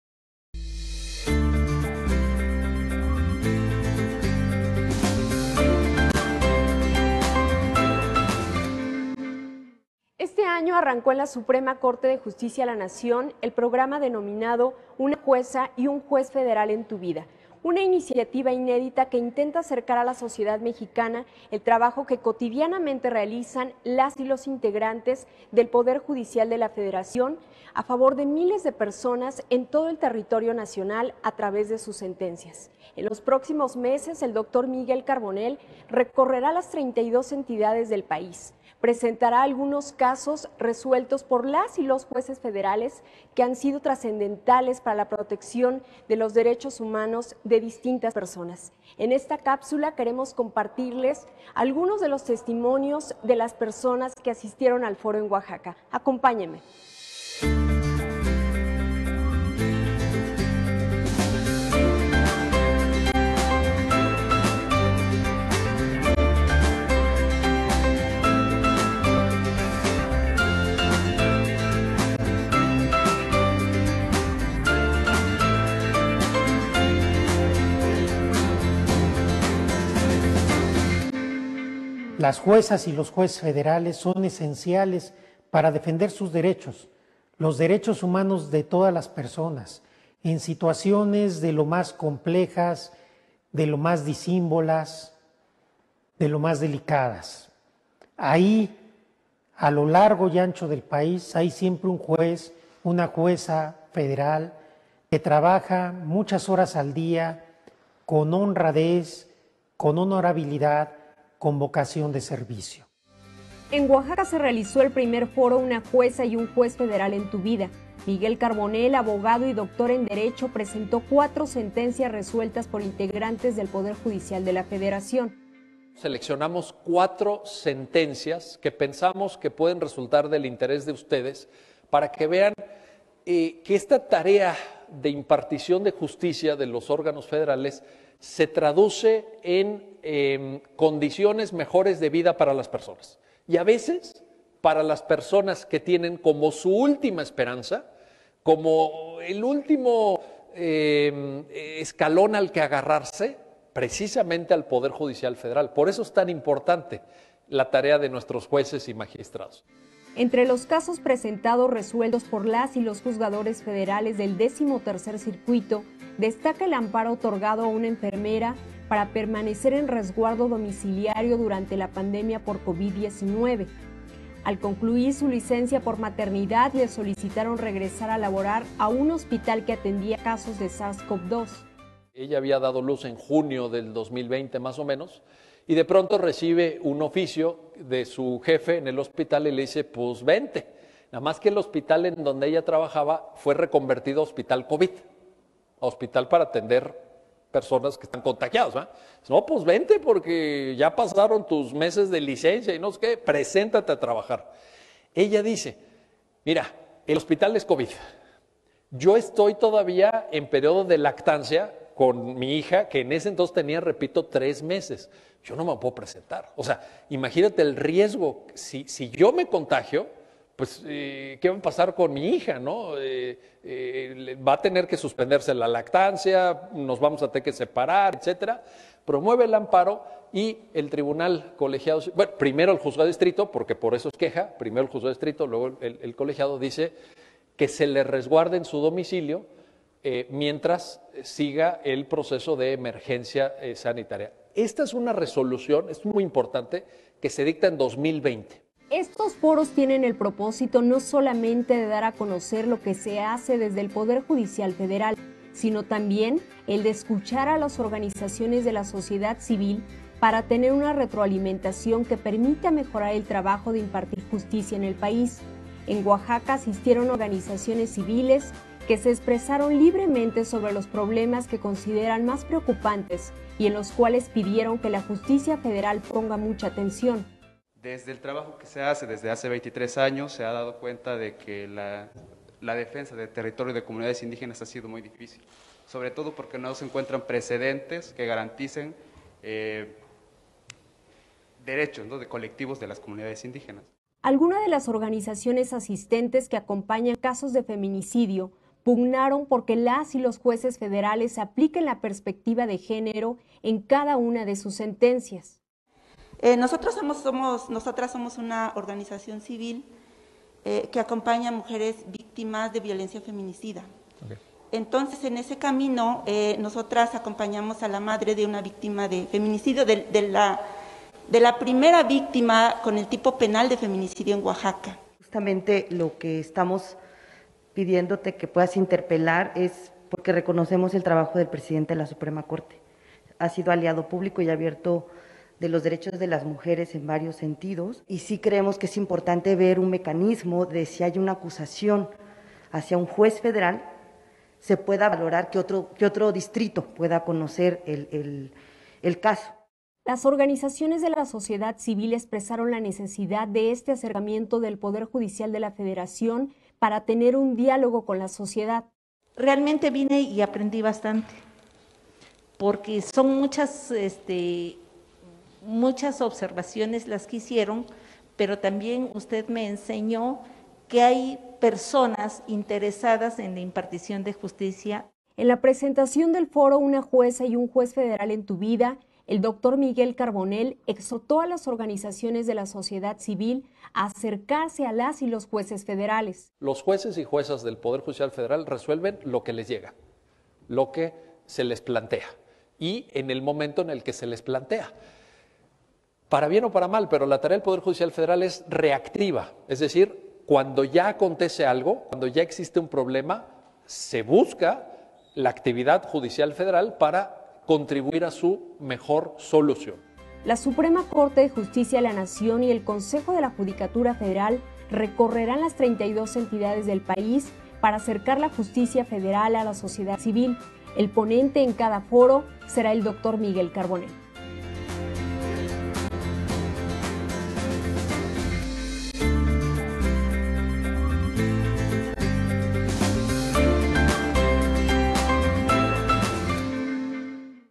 Una Jueza y un Juez Federal en tu Vida: Oaxaca
Con la participación del Dr. Miguel Carbonell.
Información de Erica Mora.